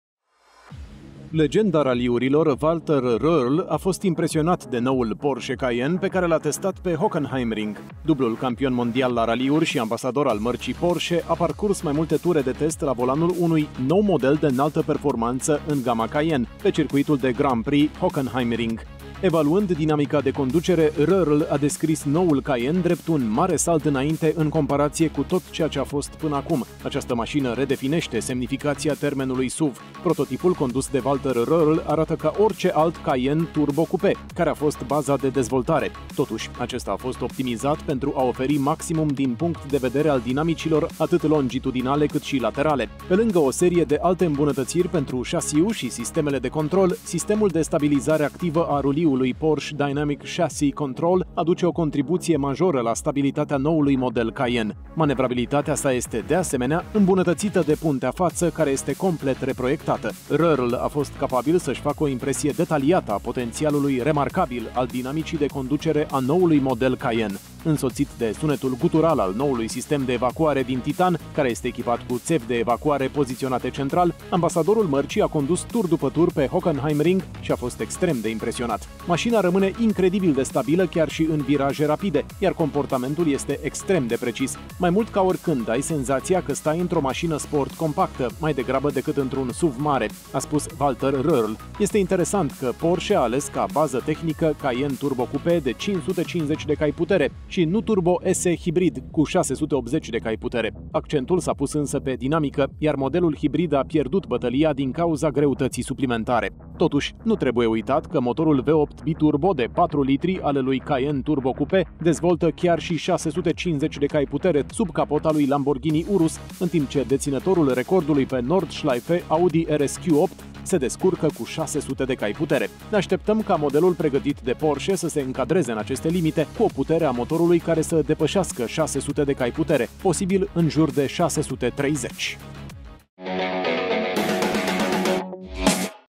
Legenda raliurilor Walter Röhrl testează noua versiune supersport Porsche Cayenne Coupé
Legenda raliurilor, Walter Röhrl, s-a declarat impresionat de noul Porsche Cayenne pe care l-a testat pe Hockenheimring. Prototipul este o versiune modificată a lui Cayenne Turbo Coupé, care anunță o nouă supersportivă în gama Porsche Cayenne. Röhrl spune că este „un mare salt înainte în comparație cu tot ceea ce a fost până acum”. Deocamdată, îl credem e cuvânt. Toate detaliile, pe